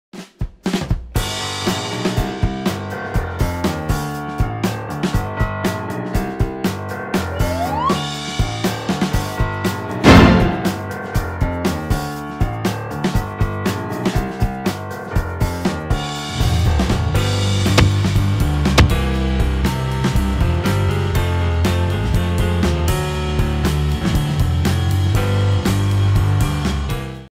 神乎其技停車法 再來一次，行嗎？
行經一般產業道路，乍看有車停放在排水溝旁，起初不以為意，仔細看就忍不住要讚嘆駕駛人的技術，原來只差不到一公分的停車法，有人猜說駕駛應該是喝醉酒？大伙兒戲稱恐怕沒辦法再來一次。